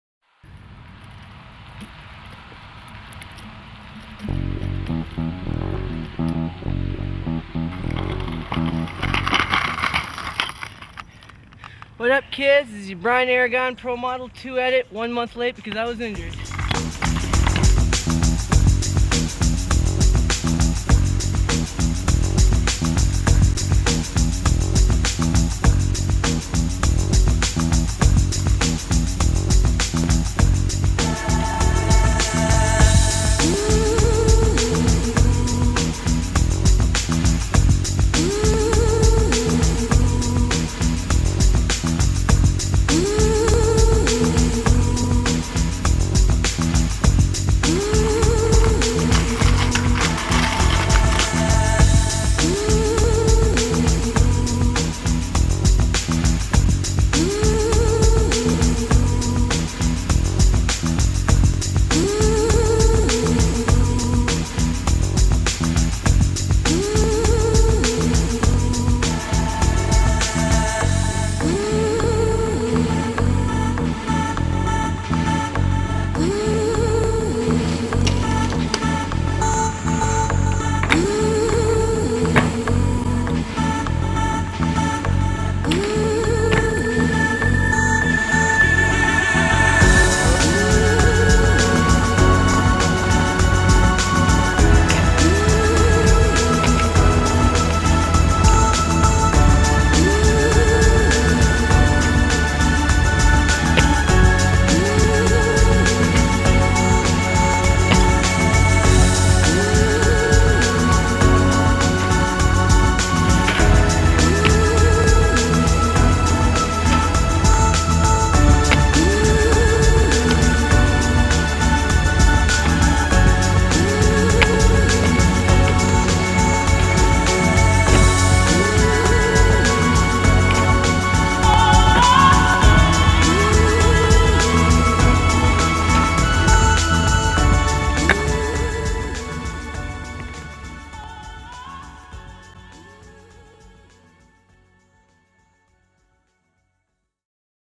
Brett Dasovic - Razors Flow rider and his Aragon2 SL's
Brett Dasovic, 21 yr old from Minnesota, been riding for Razors for a good couple of years.  Catch him testing his new jams in the snow-ta.

We are proud to have Brett as part of the family.  Thanks for the edit my boy.

Razors